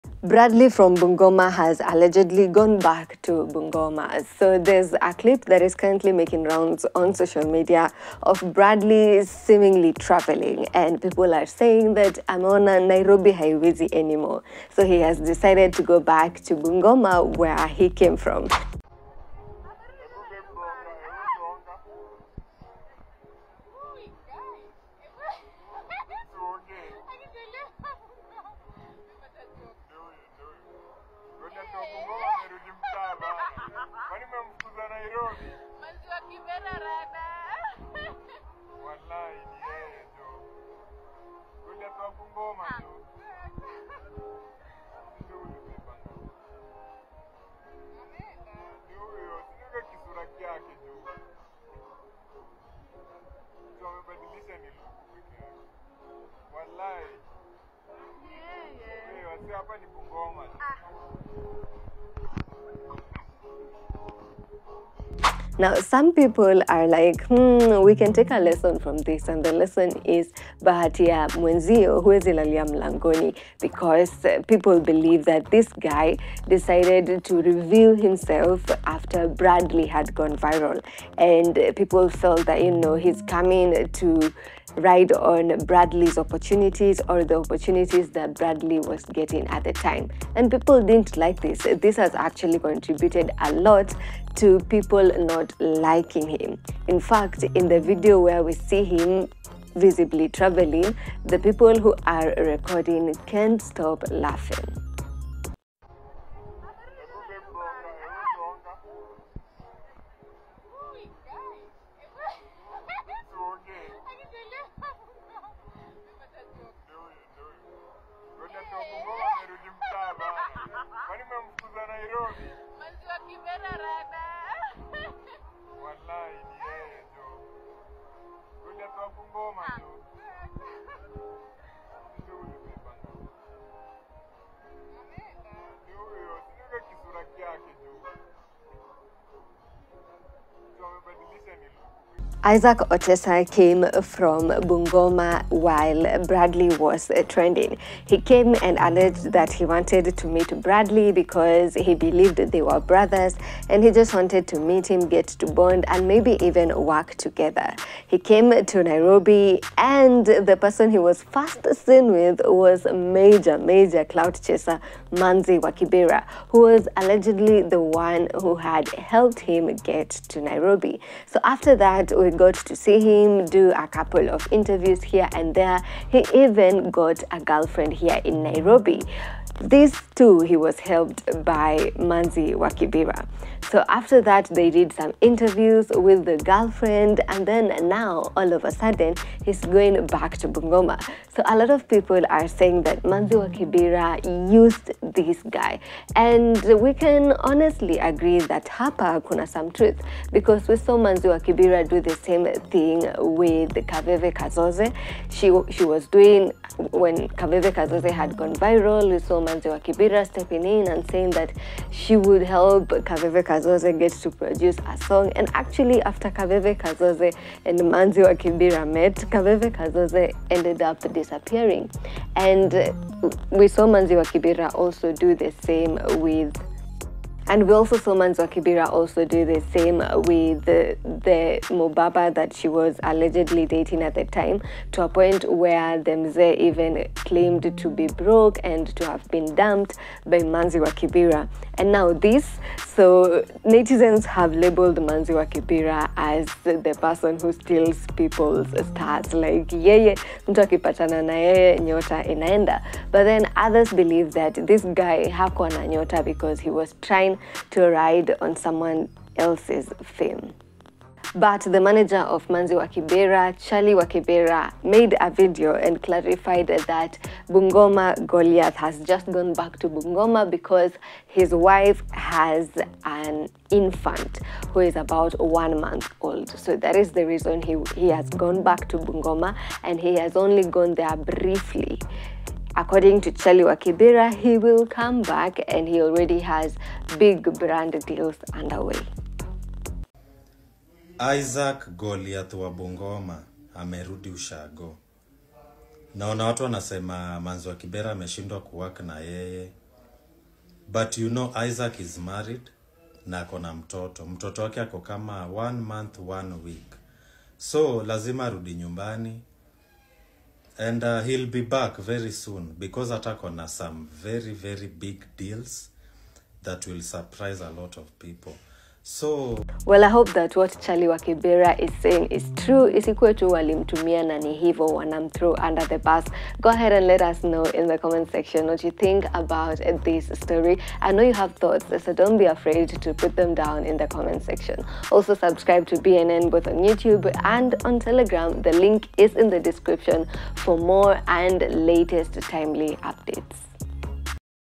Why Goliath of Bungoma, Isaac Otesa, Returned to Bungoma! - BNN
Goliath of Bungoma recently returned to Bungoma after spending a few days in Nairobi. He had traveled to Nairobi with the goal of meeting fellow tall man, Bradley Marongo. During his stay, Goliath of Bungoma participated in several interviews and even found himself a girlfriend. However, some felt that he was attempting to ride on Bradley Marongo's fame as Kenya's tallest man, a move that didn't sit well with many. Critics even predicted that Goliath of Bungoma would soon head back home to Bungoma.

Recently, Goliath of Bungoma was spotted traveling, and Chali wa Kibera confirmed that he had indeed returned to Bungoma. However, Chali wa Kibera clarified that Goliath of Bungoma had only gone back to check on his wife and their one-month-old baby, and that he would soon be returning to Nairobi. He also mentioned that Goliath of Bungoma has several brand deals lined up, urging people not to mock him for traveling to Bungoma.

Regards
Edgar Obare